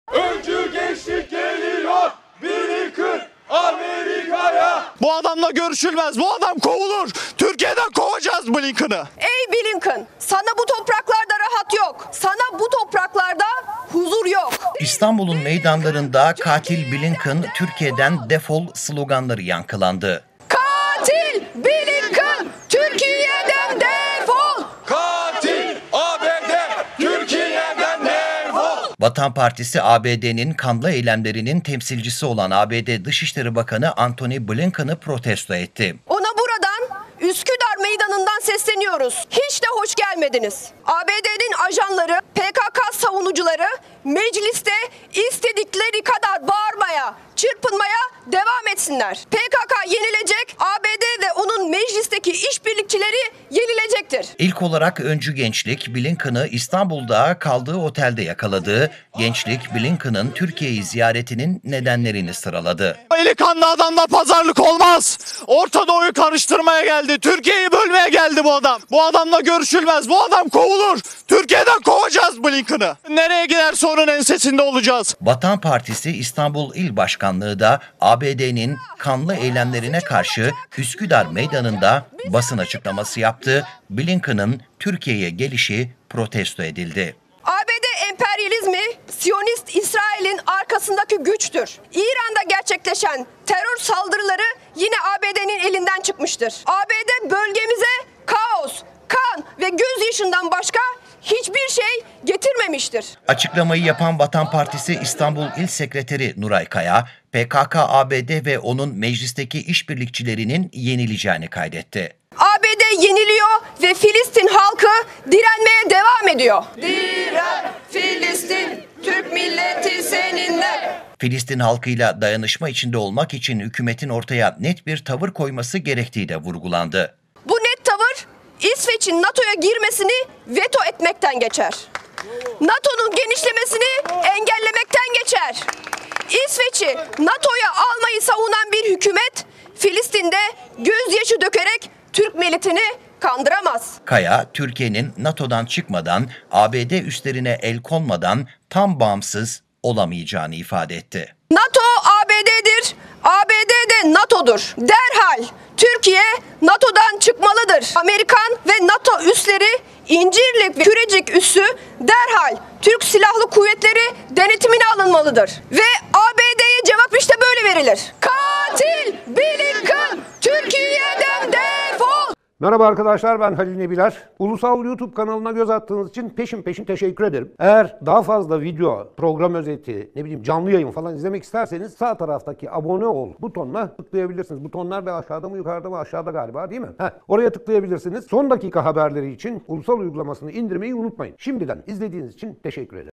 VATAN PARTİSİ'NDEN BLINKEN'A PROTESTO | Ulusal Haber
Vatan Partisi İstanbul İl Başkanlığı, ABD Dışişleri Bakanı Antony Blinken'ın Türkiye'ye gelişini protesto etti. Üsküdar meydanında yapılan açıklamada ABD ve Meclis'teki iş birlikçilerinin yenileceği kaydedildi. Türkiye'nin NATO’dan çıkmadan, ABD üslerine el konmadan tam bağımsız olamayacağı da vurgulandı.

🔴ULUSAL KANAL:
⚫SON DAKİKA HABERLERİ
⚫GÜNDEMDEKİ HABERLER